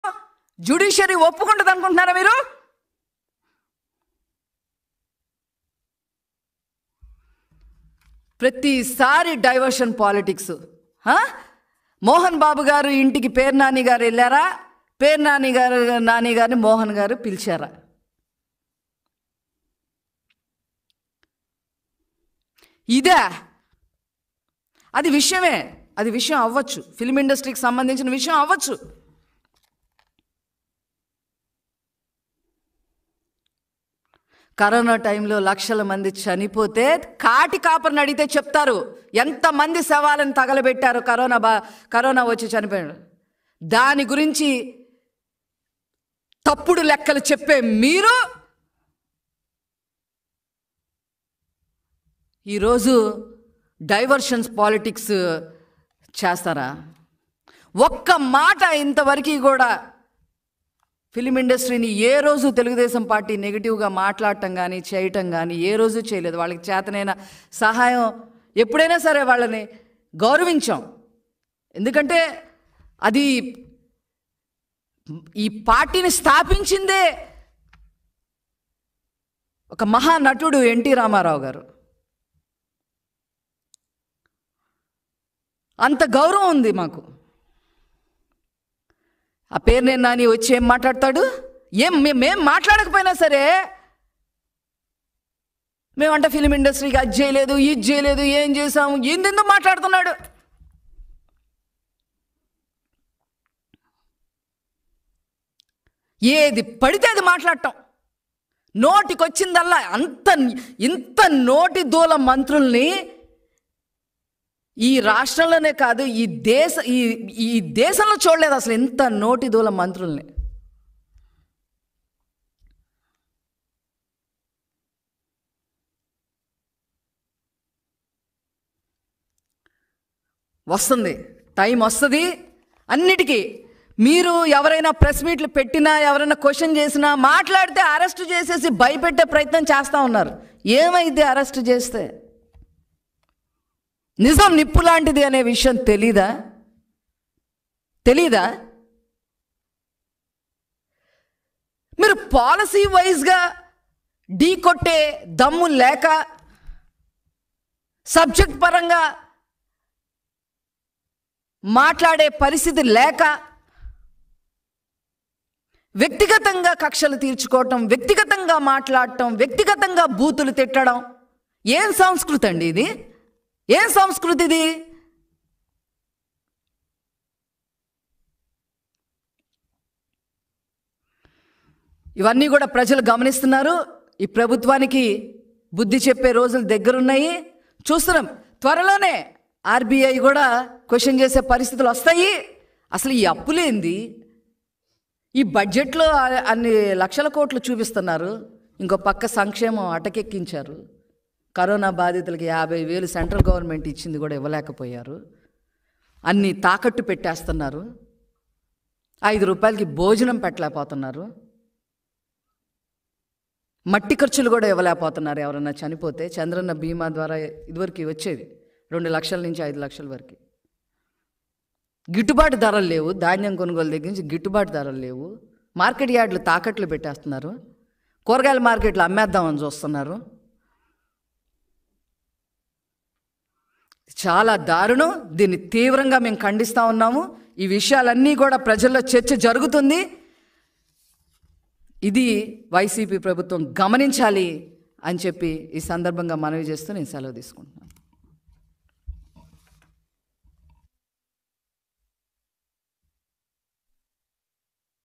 TDP Panchumarthi Anuradha Reacts On AP Cinema Ticket Issue | Mahaa News
LIVE 🔴 - పంచుమర్తి అనురాధ మీడియా సమావేశం | Mahaa News